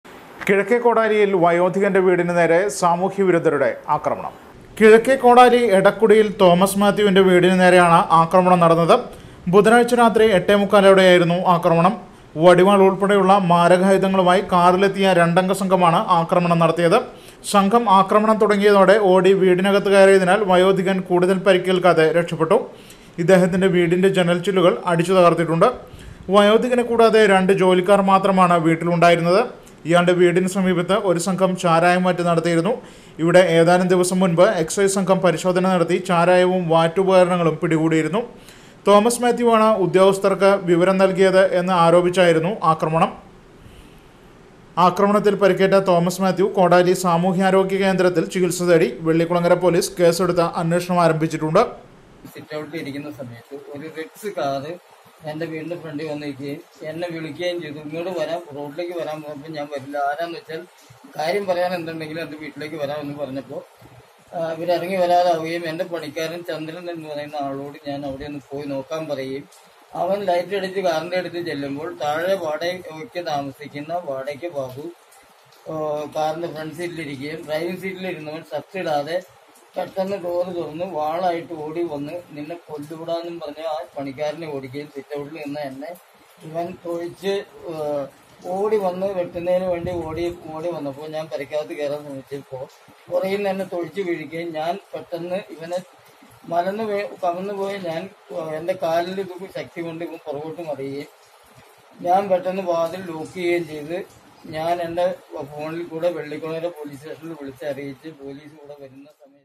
വയോധികന്റെ വീടിനു നേരേ സാമൂഹ്യ വിരുദ്ധരുടെ ആക്രമണം.
വയോധികന്റെ വീടിനു നേരേ സാമൂഹ്യ വിരുദ്ധരുടെ ആക്രമണം. കിഴക്കേ കോടാലി
എടക്കുടിയിൽ തോമസ് മാത്യുവിന്റെ വീടിന് നേരെയാണ് ആക്രമണം നടന്നത്.